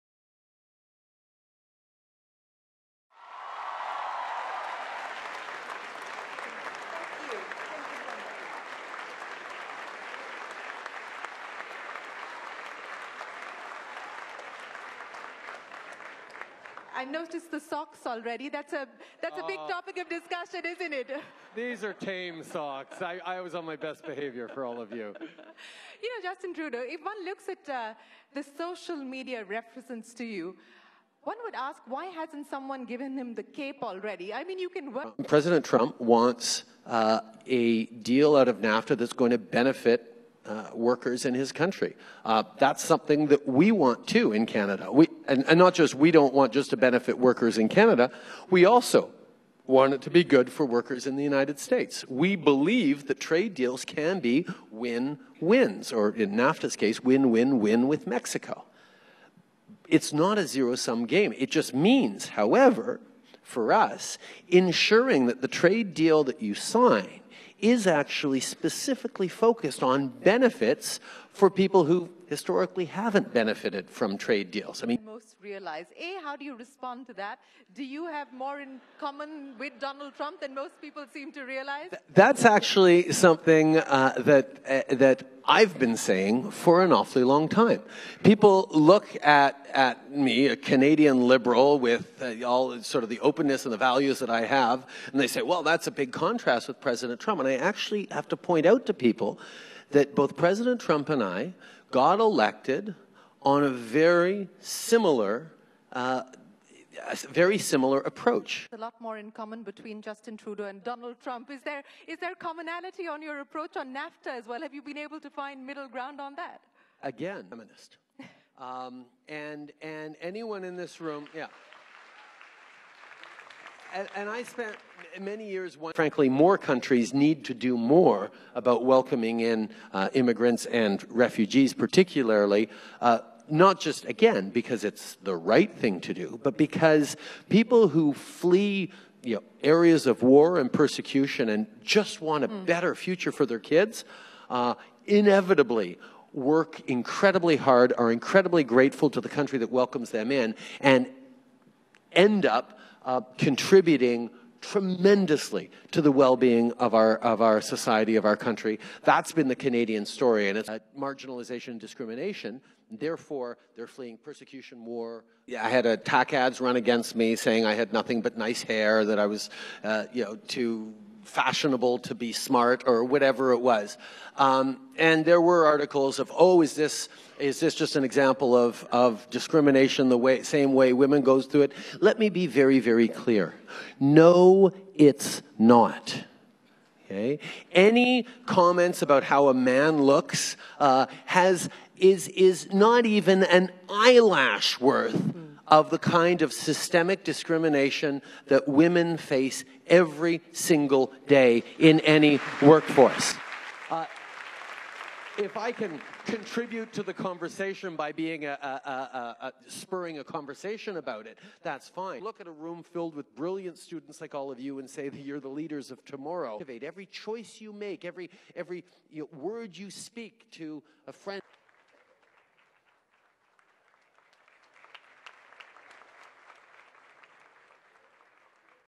20 Feb, 2018 - Canadian PM Trudeau feels NAFTA deal is a win-win situation for USA, Mexico
20 Feb, 2018 (ANI) - Canadian PM Trudeau feels NAFTA deal is a win-win situation for USA, Mexico